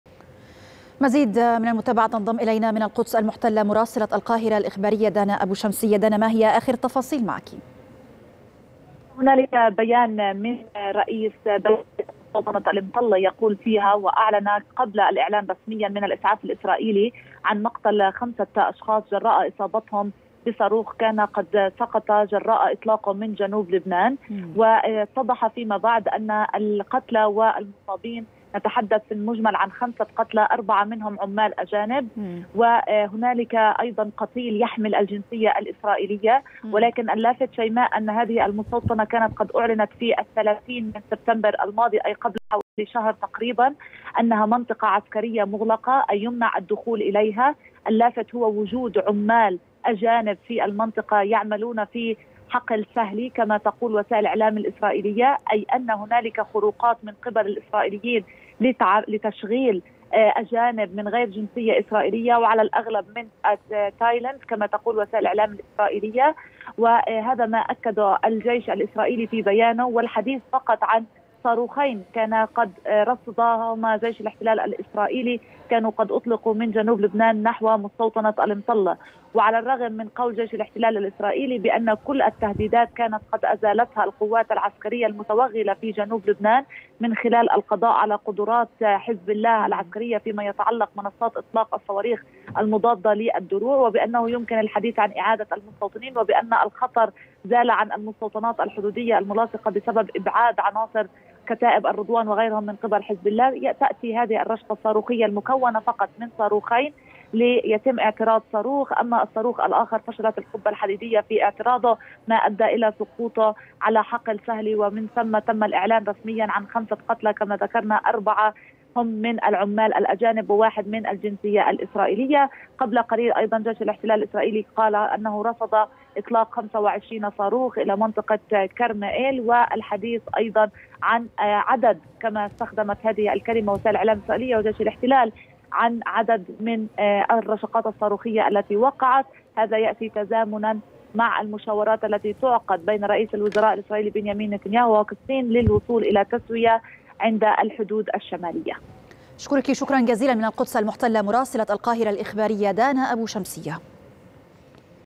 صواريخ حزب الله تصيب الداخل الإسرائيلي.. قتلى وإصابات خطيرة | القصة الكاملة مع مراسلتنا
31/10/2024